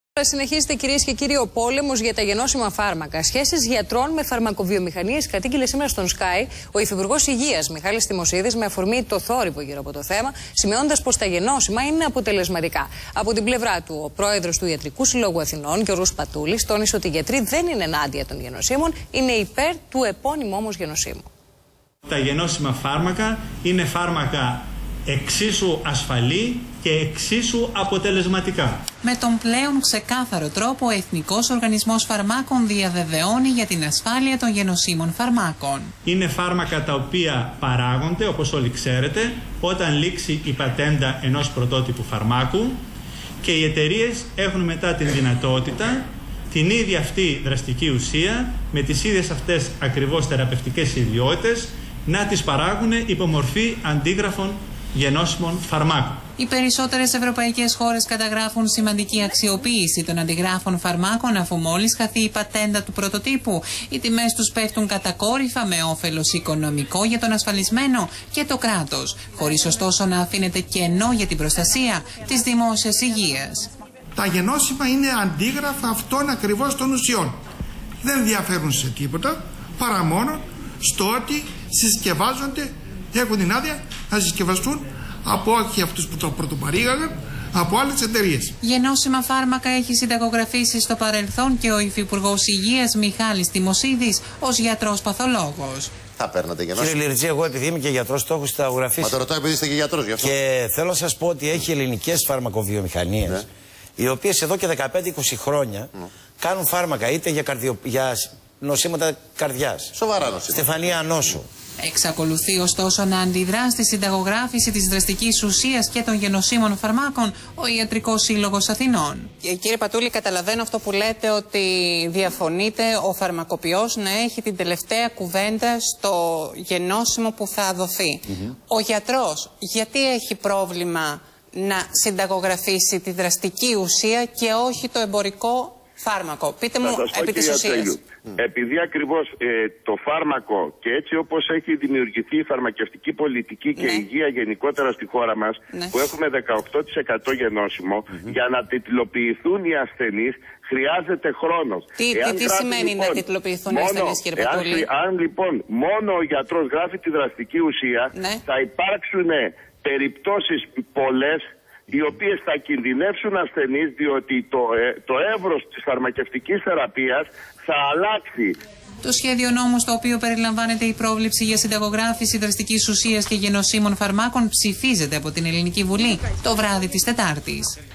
O Γ.Πατούλης στον SKAI για την δραστική ουσία. 28-2-2012
Σημεία συνέντευξης Δημάρχου Αμαρουσίου και Προέδρου Ι.Σ.Α. κ. Γιώργου Πατούλη στο δελτίο ειδήσεων του καναλιού SKAI για την δραστική ουσία.28-2-2012